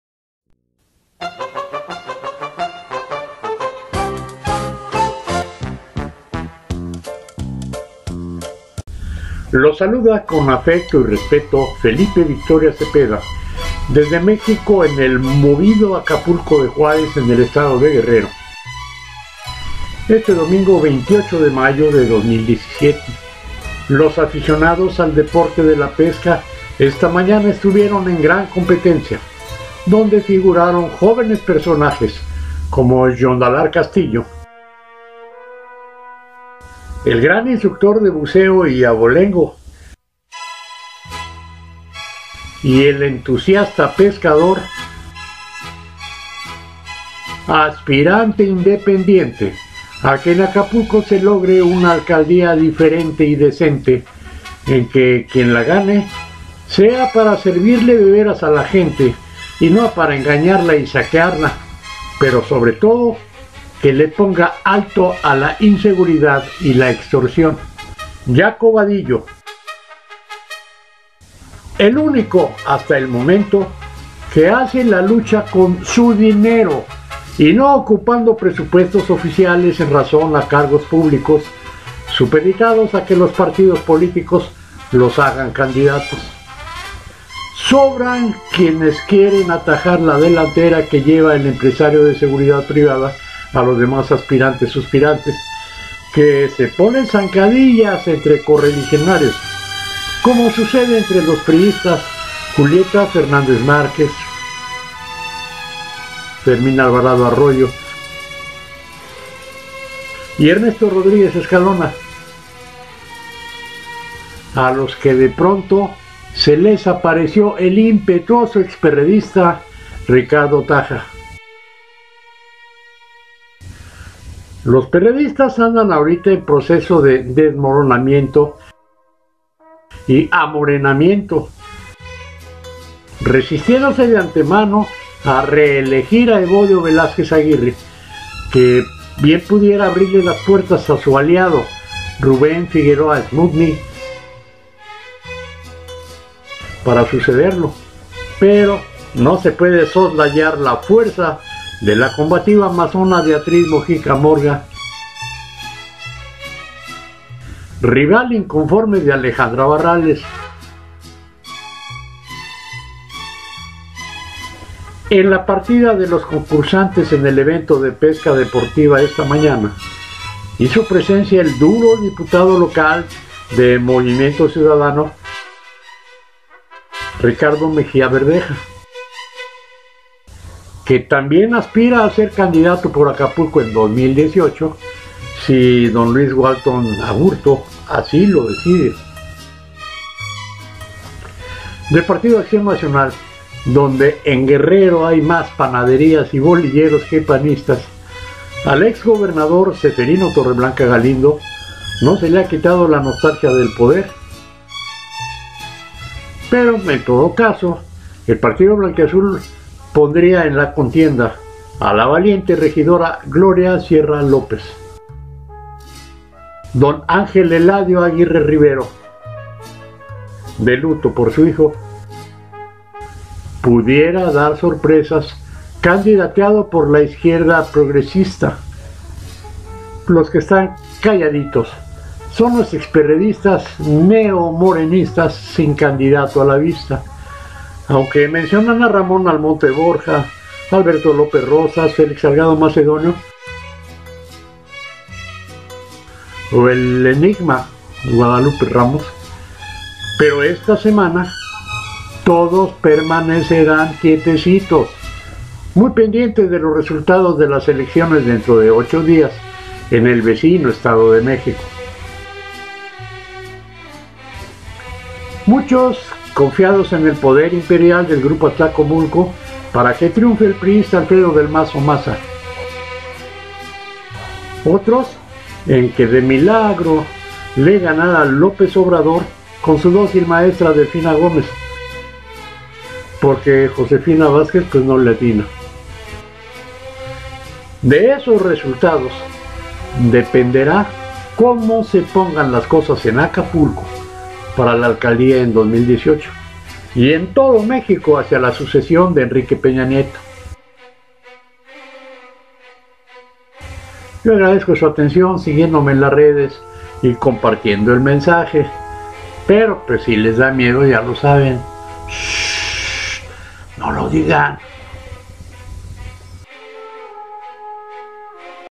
domingo 28 mayo de 2017 eleccsmex 0002
¿Cuenta regresiva en el Estado de México para el estallido preliminar y guerra fría en Acapulco por la Alcaldía?